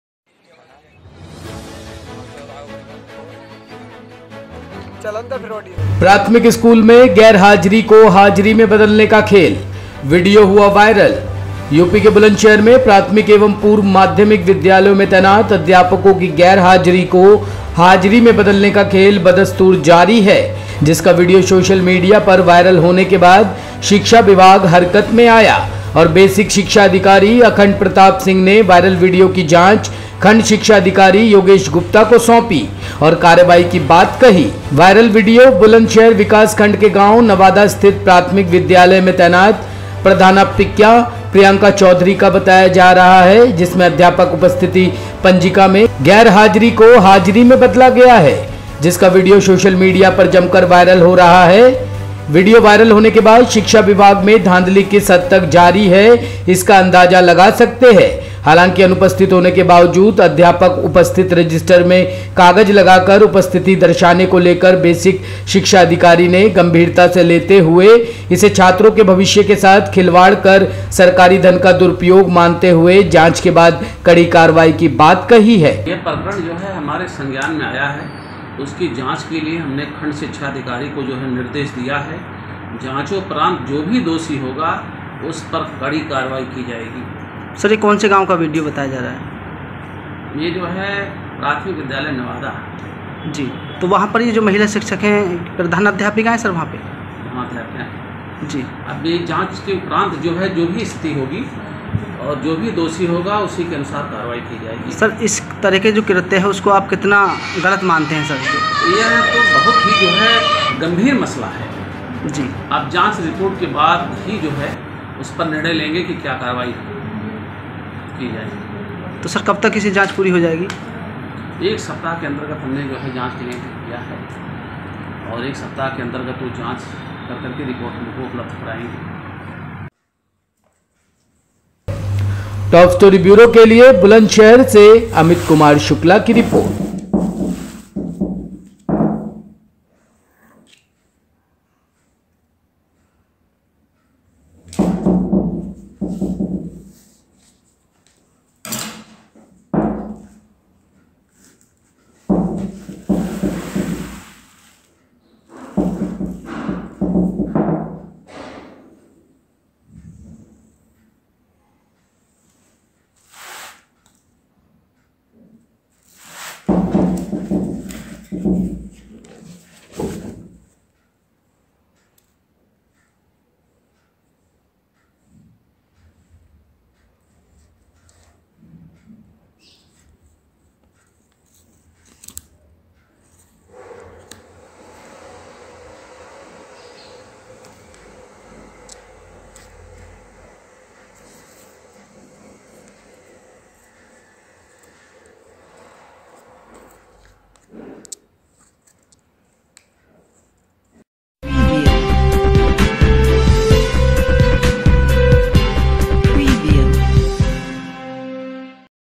Bulandshahr, UP: प्राथमिक स्कूल में गैरहाजिरी को हाजिरी में बदलने का खेल, वीडियो वायरल | Top Story
यूपी के बुलंदशहर में प्राथमिक एंव पूर्व माध्यमिक विद्यालयों में तैनात अध्यापकों की गैरहाजिरी को हाजिरी में बदलने का खेल बदस्तूर जारी है। जिसका वीडियो सोशल मीडिया पर वायरल होने के बाद शिक्षा विभाग हरकत में आया और बेसिक शिक्षा अधिकारी अखंड प्रताप सिंह ने वायरल वीडियो की जांच खण्ड शिक्षा अधिकारी योगेश गुप्ता को सौंप कारवाई की बात कही है।